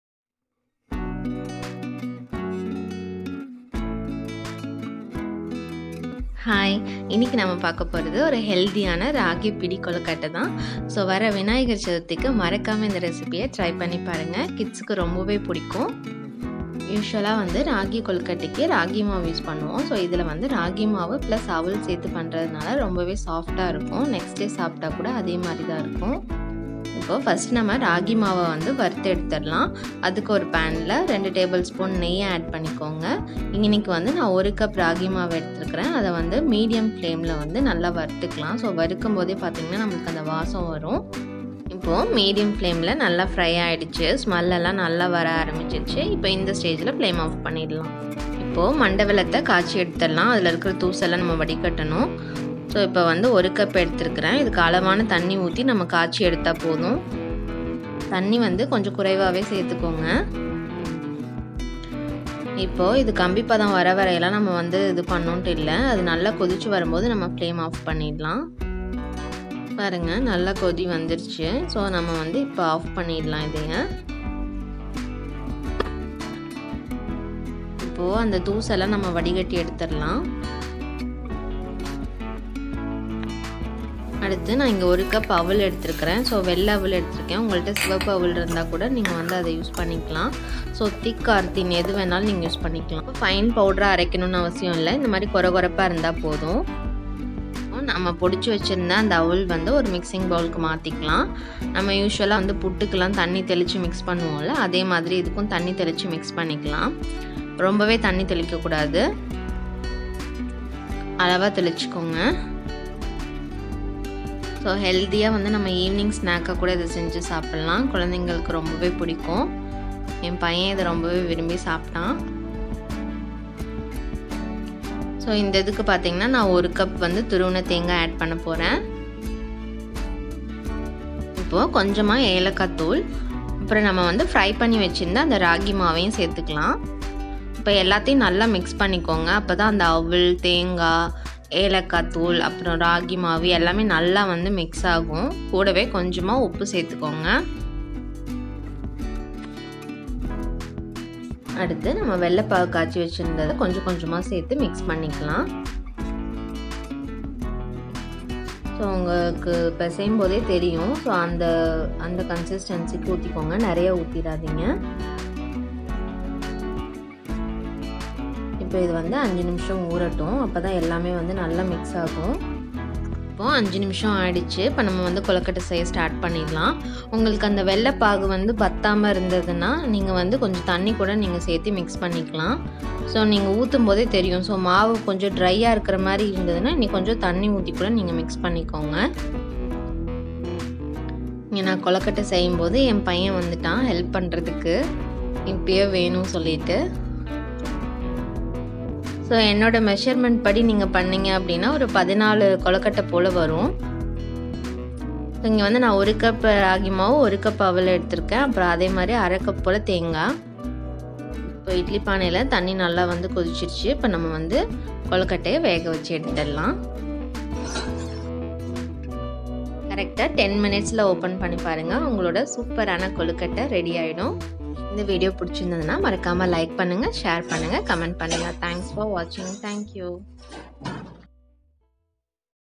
சத்தும் சுவையும் நிறைந்த கேழ்வரகு இனிப்பு கொழுக்கட்டை |Kozhukattai in tamil|Healthy Evening Snack
Ragi kozhukattai/Finger Millet dumpling is a calcium-rich steamed millet sweet.
Millet Dumplings are very easy and filling after-school snacks. Try making this Ragi kozhukattai and get addicted to it.

Take a trial today and impress lord Ganesha on his Birthday !!

Ingredients

1cup ragi flour
2tsp ghee
1 cup poha/Aval
1 cup jaggery
1tsp cardamom powder
1 cup grated coconut
Salt -pinch

NOTES

1.Do not boil jaggery to thick syrup. It should just be thin as water. Immediately switch off when jaggery completely dissolves in water.
2.The softness of dumpling depends on the dough. If you want hard, use lil water in making jaggery syrup and make the dough stiff. If you want very soft dumplings, use the given measurement for water and sprinkle more if required.